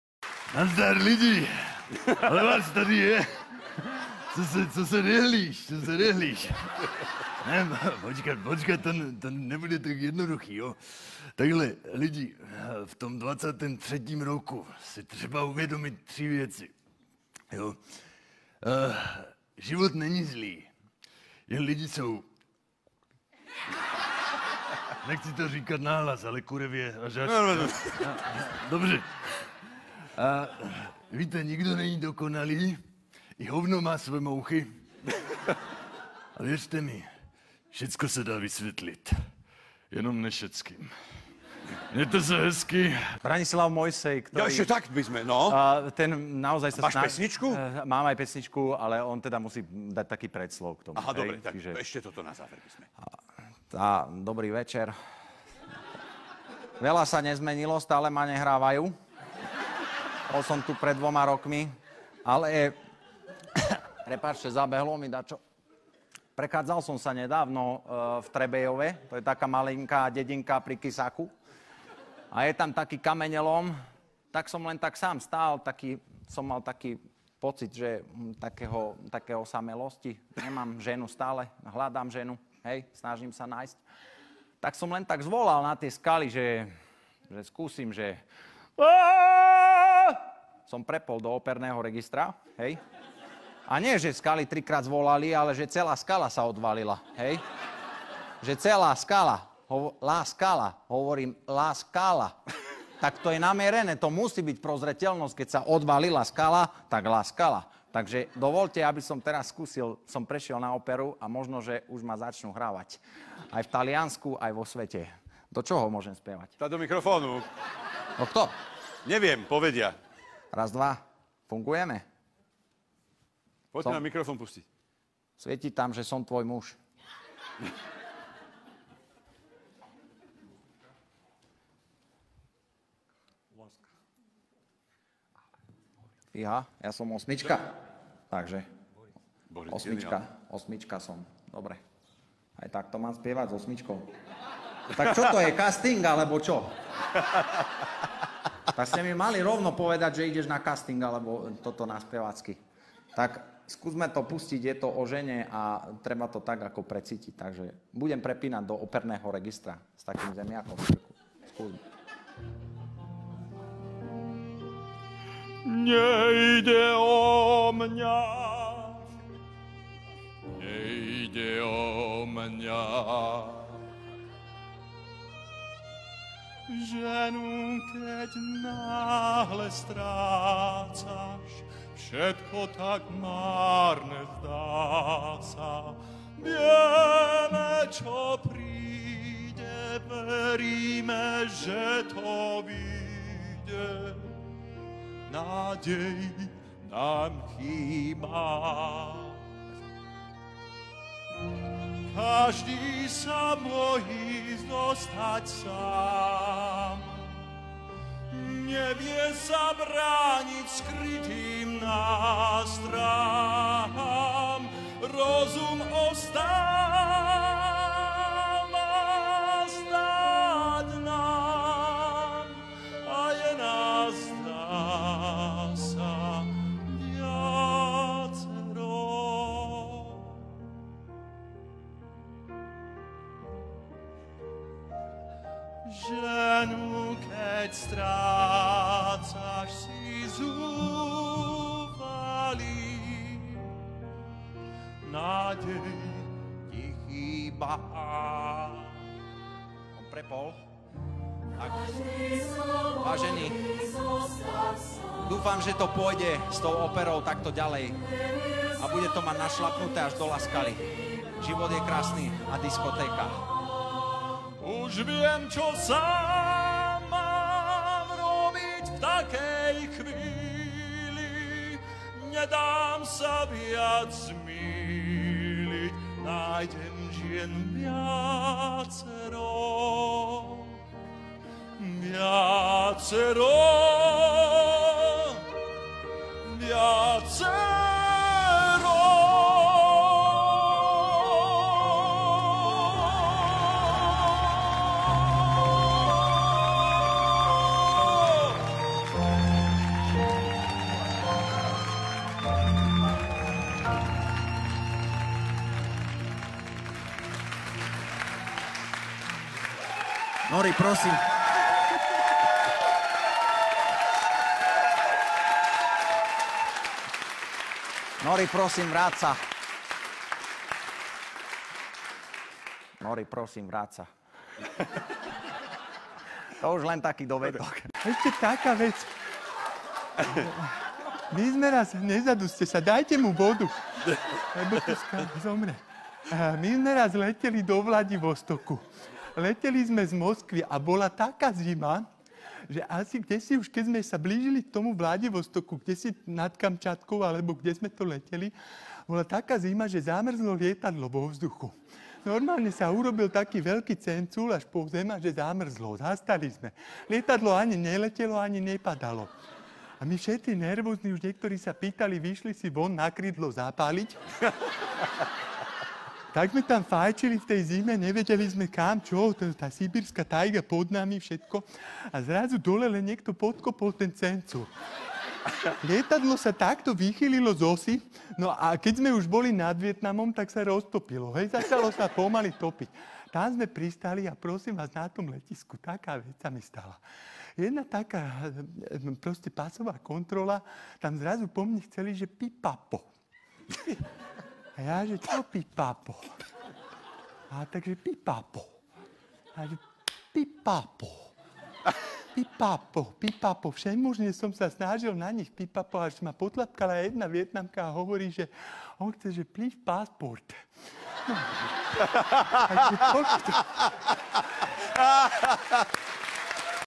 NEODVYSIELANÉ: Juraj Ďuriš s hlasom Bolka Polívku? Ide mu to perfektne, to musíte vidieť!!!
Skvelý imitátor Juraj Ďuriš pridal do svojho portfólia aj hlas Bolka Polívku a ide mu to naozaj skvelo.

Sledovať v TV nás môžete na Jednotke (RTVS): premiéra býva v SOBOTU, zvyčajne okolo 22.00